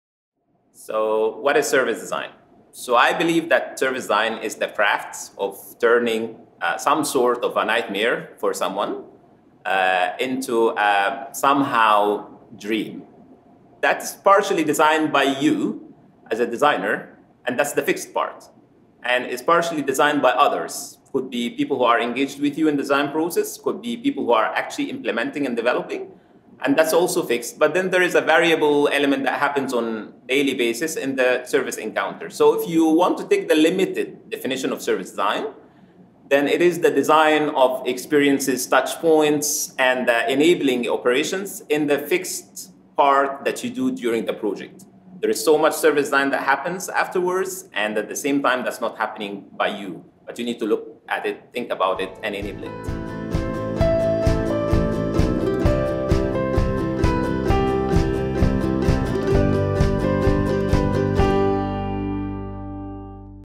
What is Service Design / Mahmoud Abdelrahman
Service Design is all about  turning someones nightmare into a dream! And remember there is so much service design happening that isn't done by you.

Learn how to get clients, colleagues, managers, CEOs and even grandmas as excited about service design as you are!